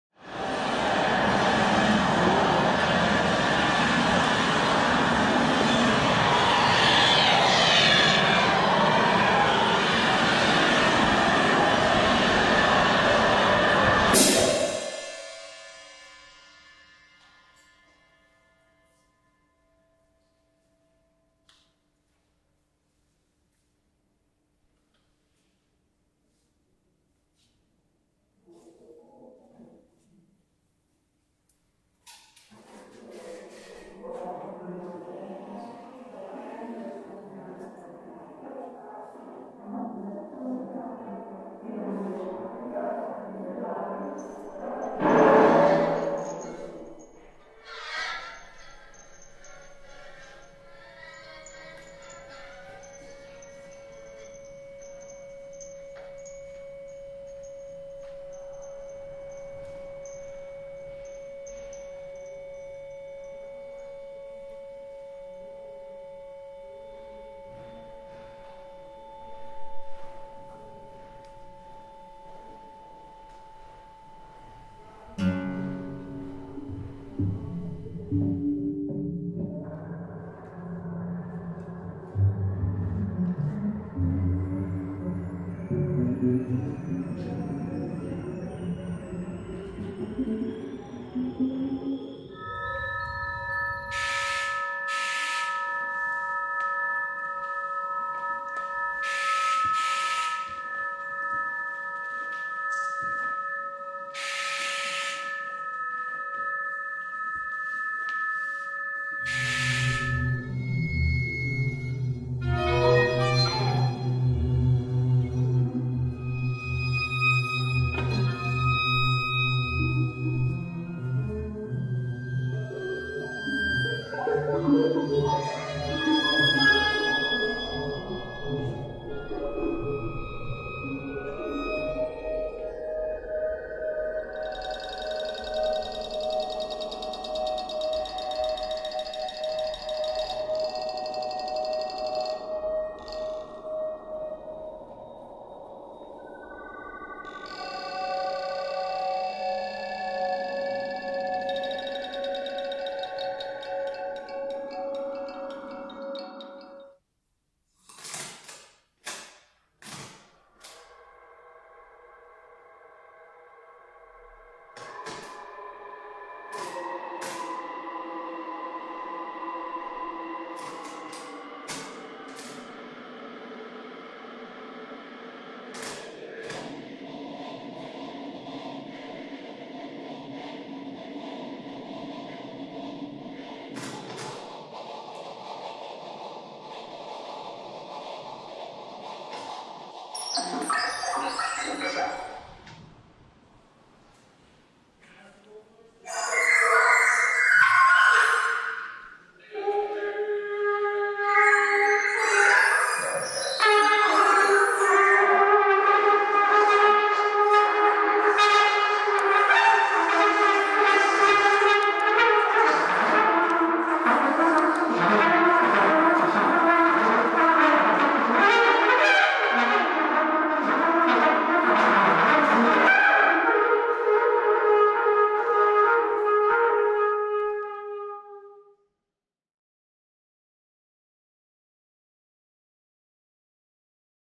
OPEN CIRCUIT 2019: Highlights
Highlights of Open Circuit 2019 on 1 September 2019 (Orpheus Institute, Ghent).

Artist-researchers of the group Music, Thought and Technology (MTT) at the Orpheus Institute filled the building with new music and sound art. They explored the ways in which technology uses architectural space and opens up new areas between installation and performance, composition and improvisation, presence and absence.